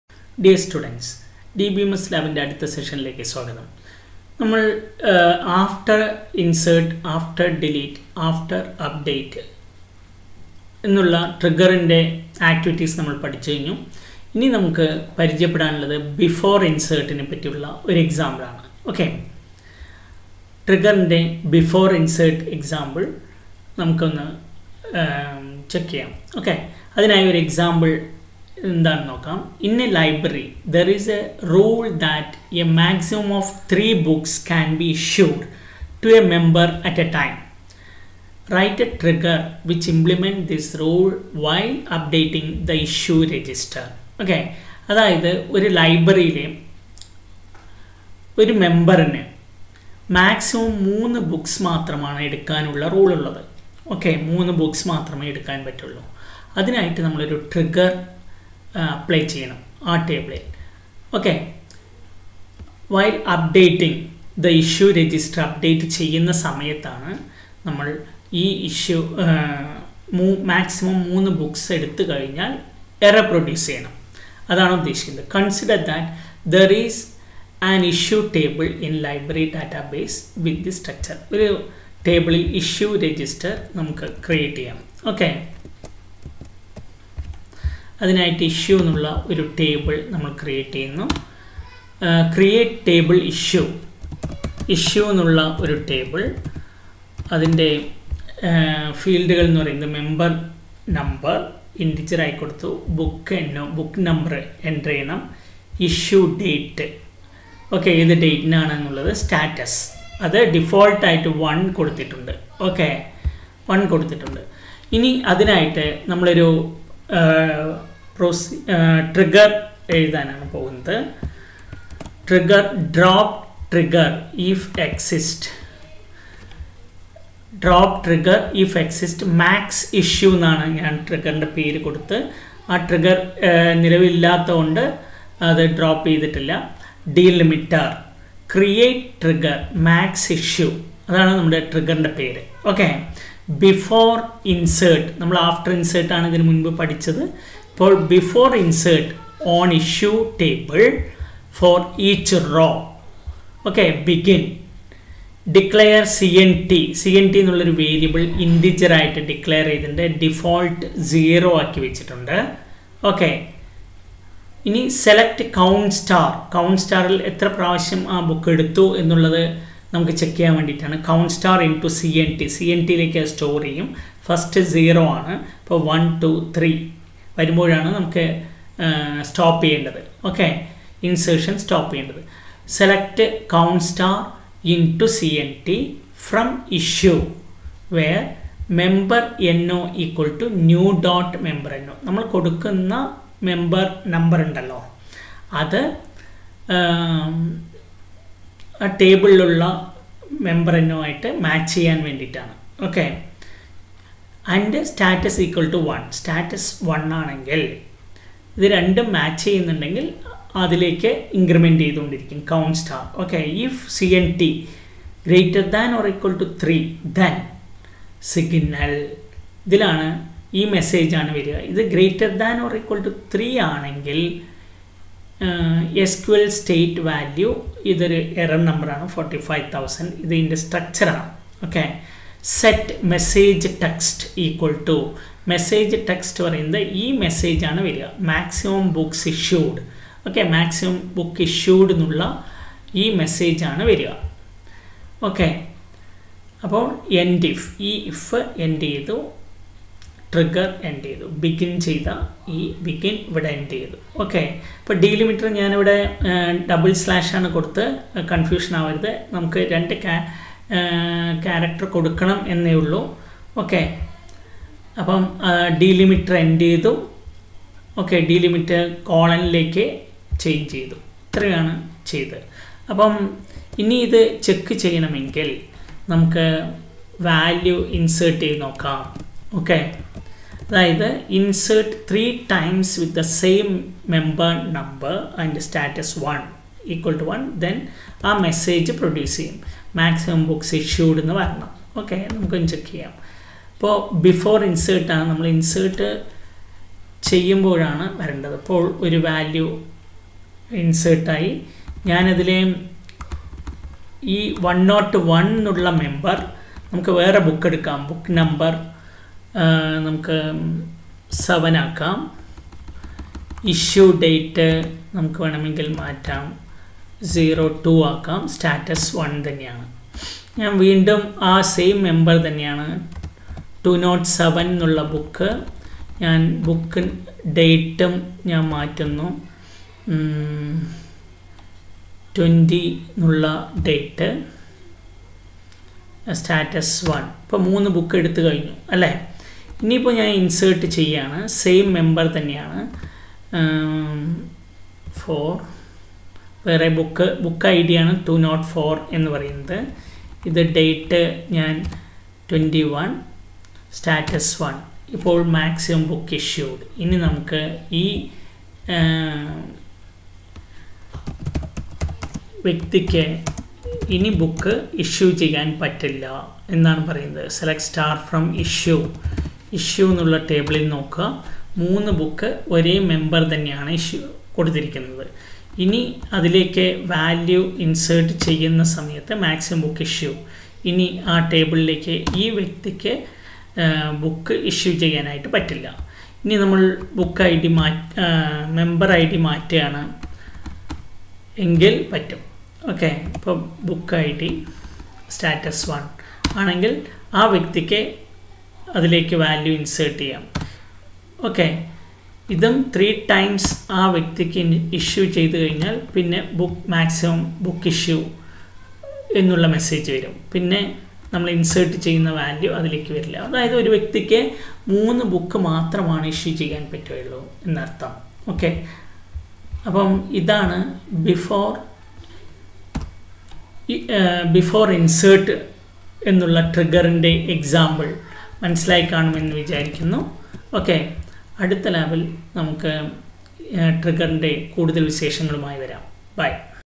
Trigger before insert - Video -32
In a Library , there is a rule that a maximum of 3 books can be issue to a member a time. Write a trigger, which implements this rule which updating issue register.

Consider that there is issue table in Library database with the structure
(memberno,bookno,issuedate,duedate, status default =1)